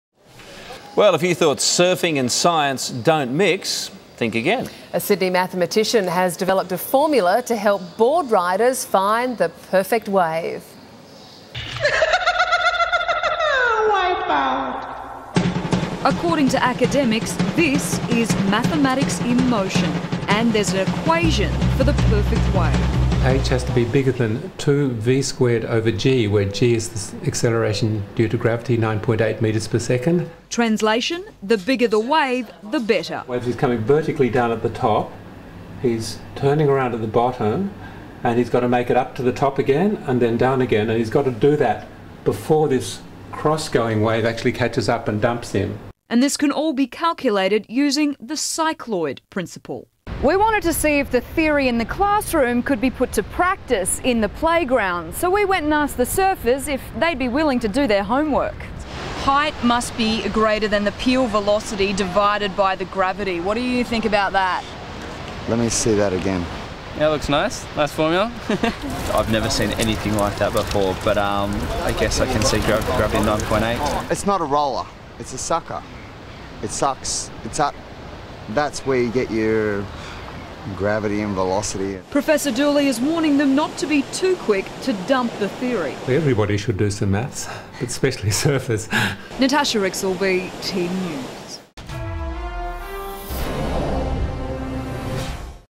Surf Science
Surfers may never be disappointed again, after a Sydney mathematician developed a formula to find the perfect wave.